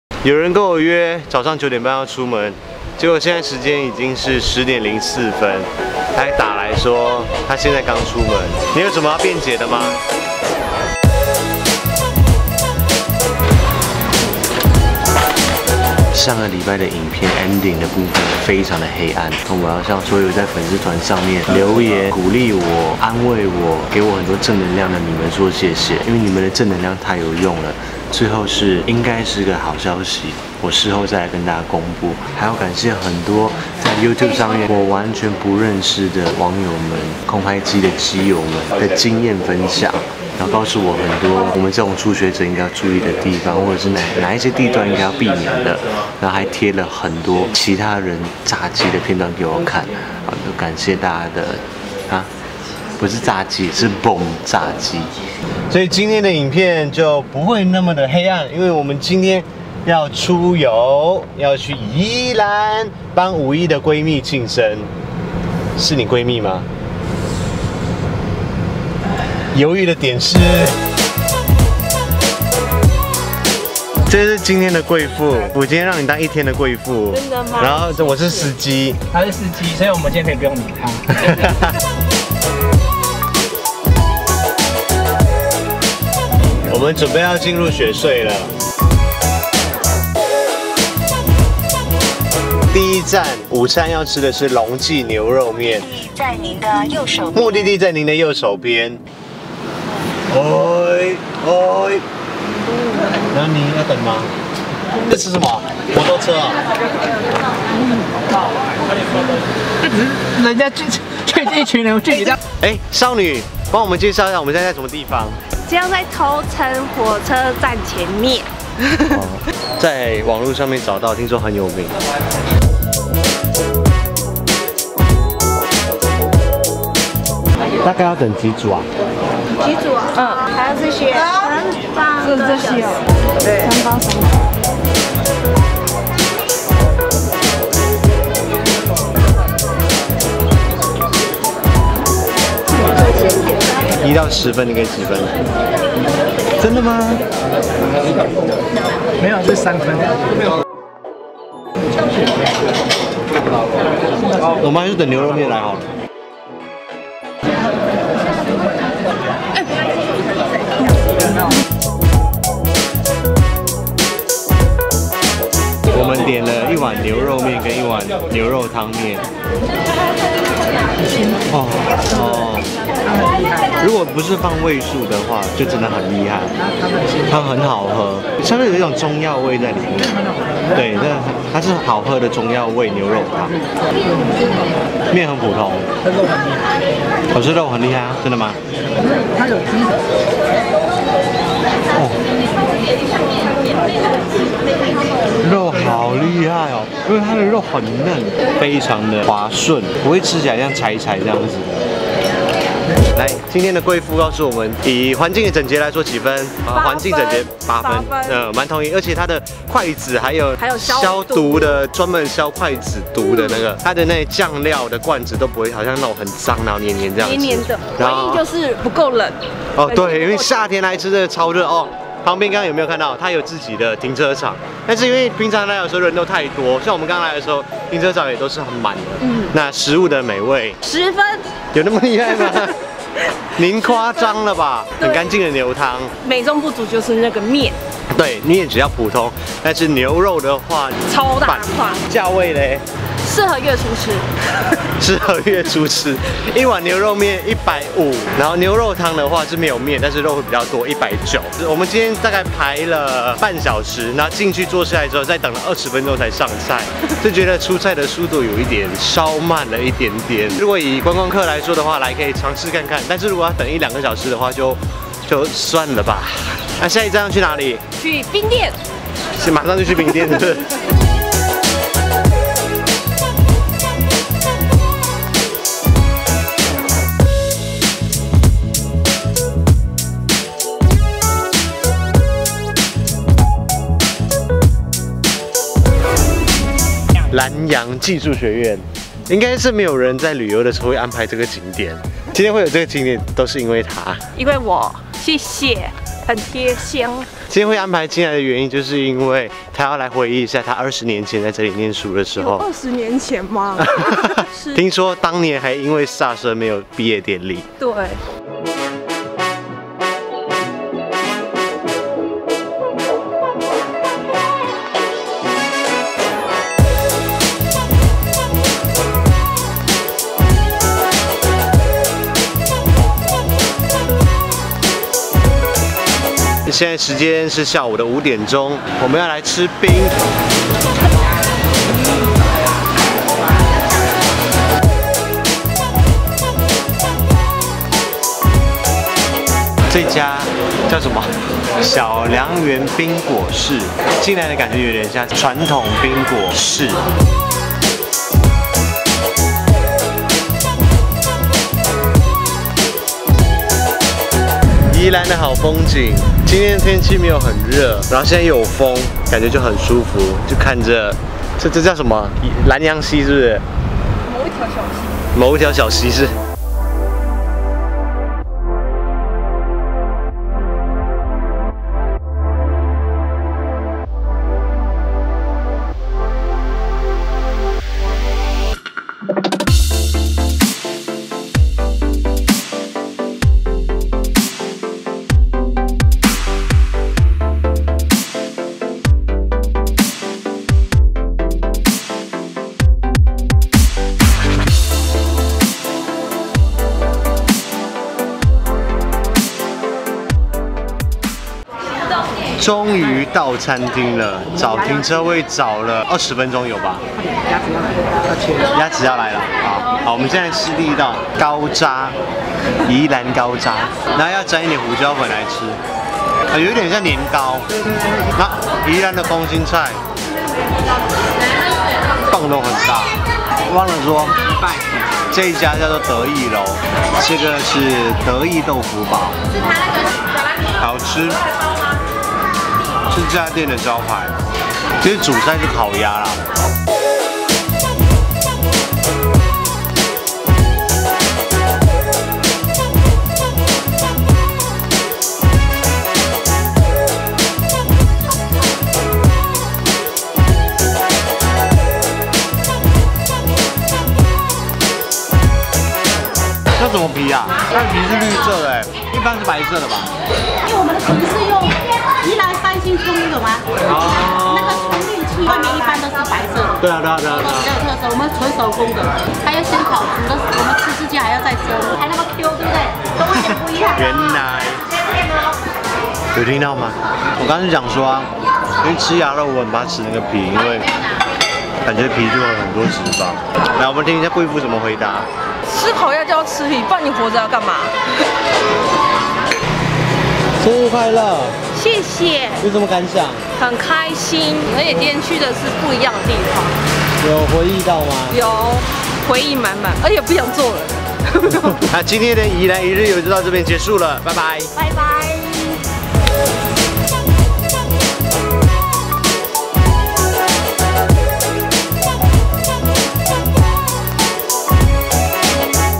宜蘭一日遊 / Alvinist Vlog 025 （中文字幕請打開 CC）
這不是一個推薦景點旅遊 Vlog。
而是一個，帶著女孩去尋找回憶的記錄。
Amy，生日快樂！